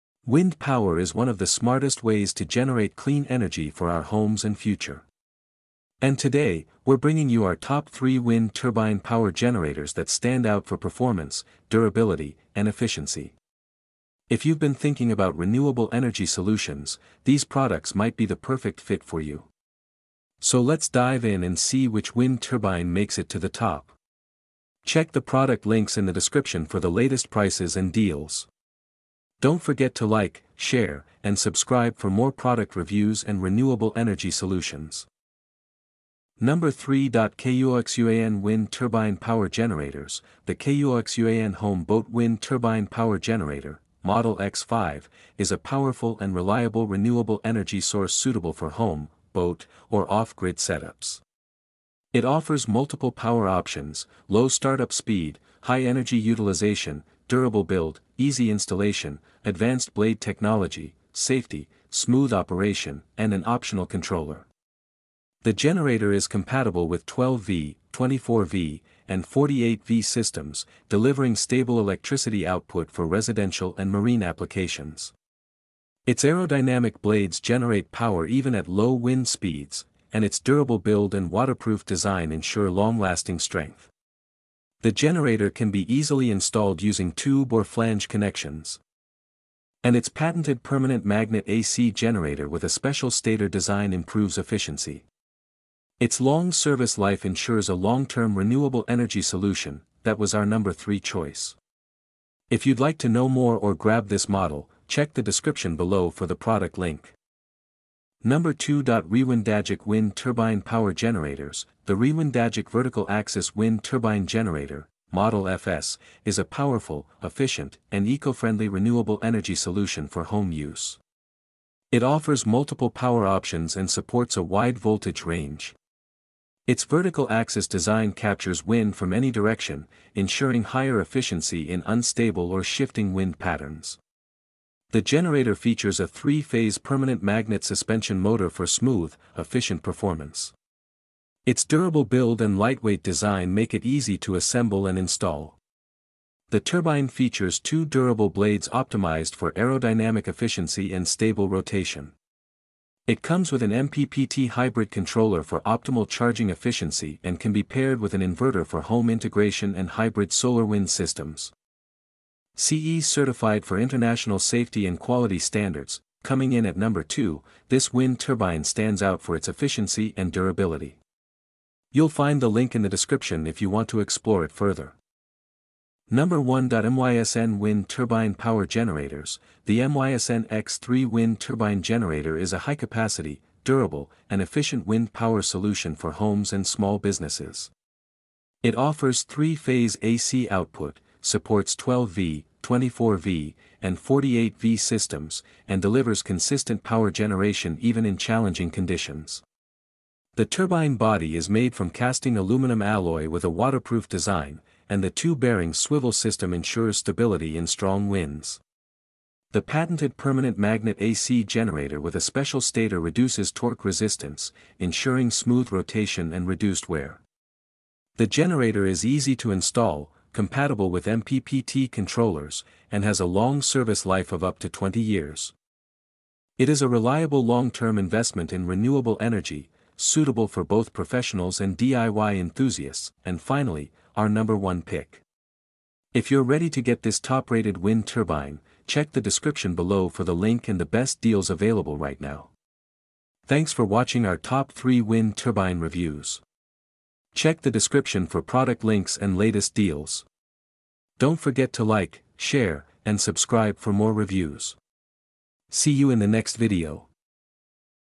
Top 3 Wind Turbine Power Generators 2025|⚡Best Renewable Energy Solutions
Looking for the best Wind Turbine Power Generators in 2025?In this video, we review our top 3 picks that offer great value and performance!
Number 3.KUOXUAN Wind Turbine Power Generators
Number 2.Rewindagic Wind Turbine Power Generators
Number 1.MYSN Wind Turbine Power Generators